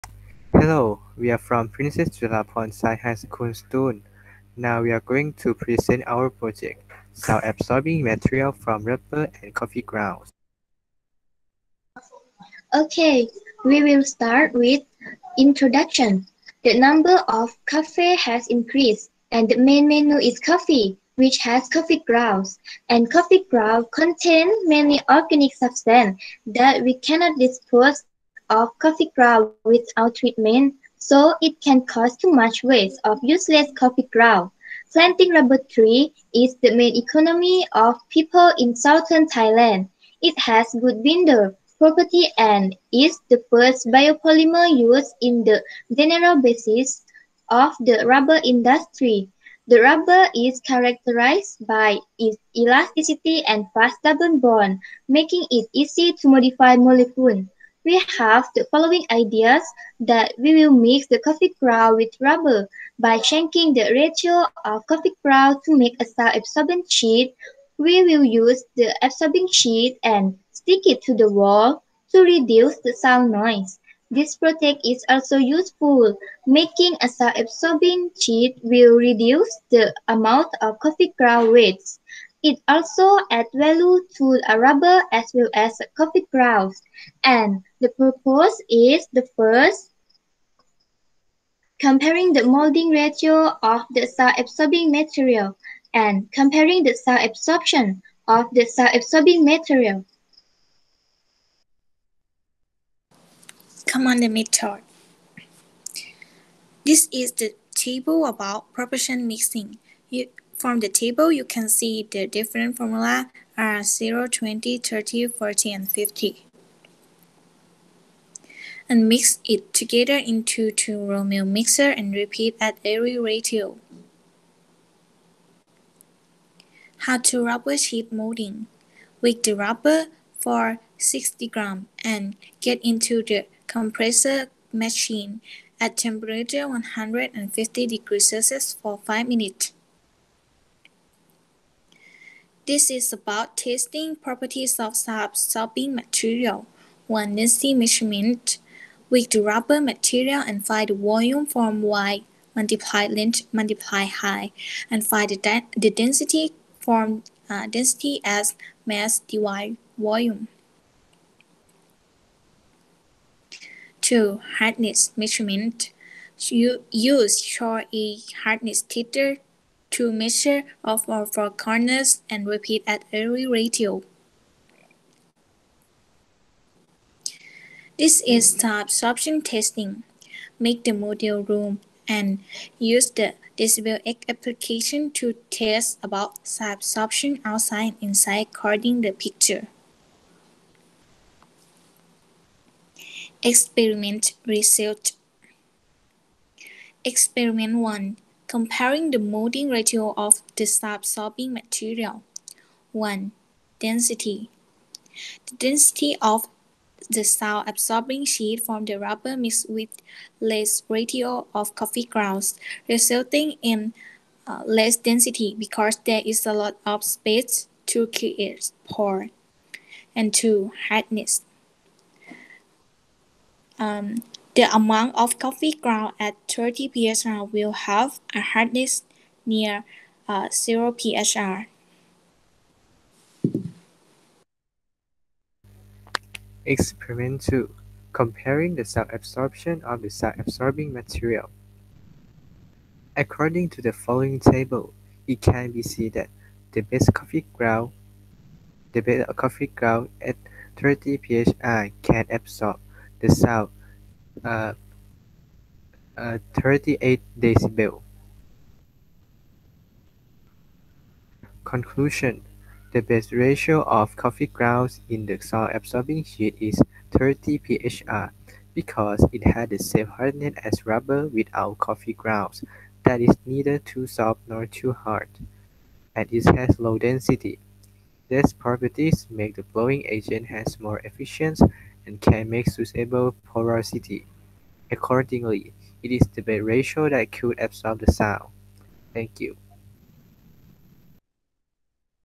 WISF2021_LS-HS-019_Sound Absorbing Material from Rubber and Coffee Grounds
Video Presentation of Fareeya Hemma, Nisita Nikuno and Thuchchakorn Thiangbulkul from Princess Chulabhorn Science High School, Thailand for World Innovative Science Fair 2021

Project ID: LS-HS-019
Title: Sound Absorbing Material from Rubber and Coffee Grounds

Give this video likes for support the research!